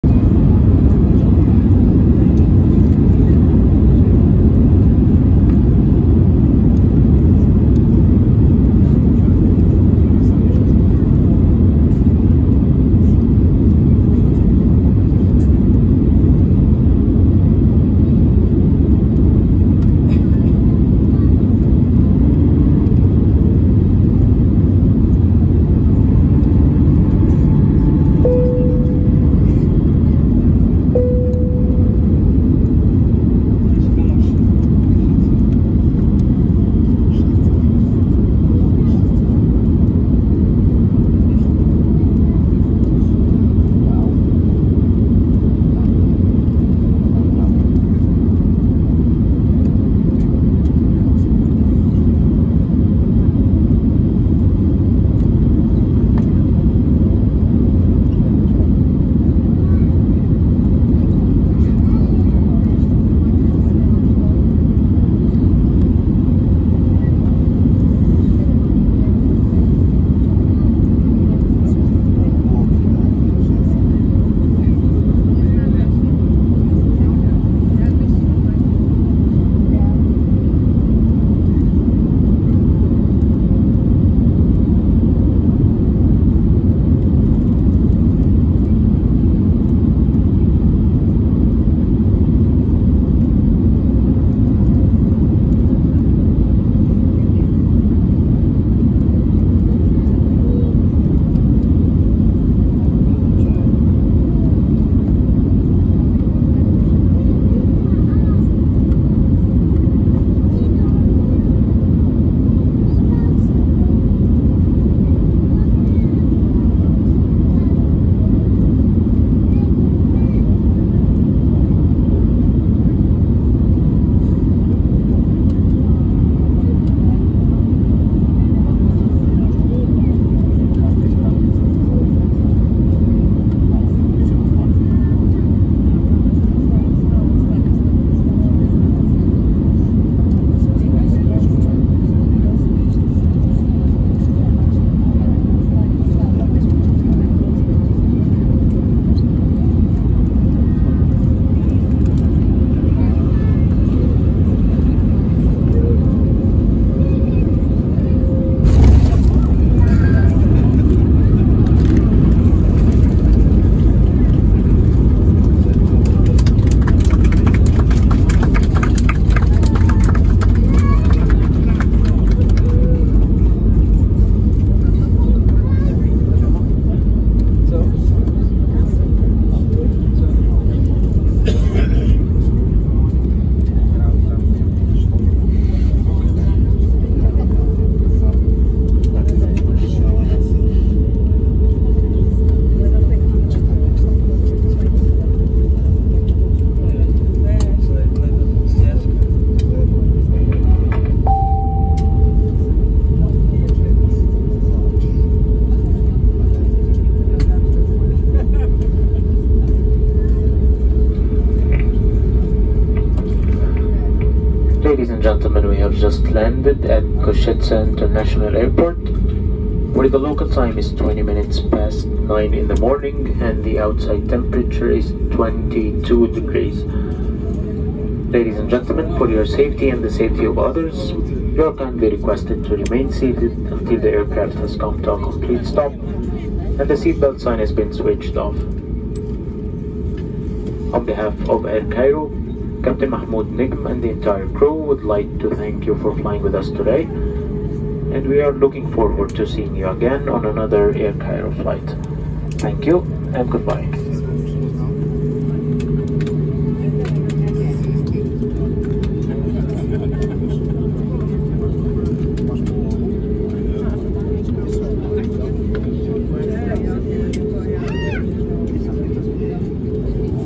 Air Cairo Airbus A320-251N Landing on Kosice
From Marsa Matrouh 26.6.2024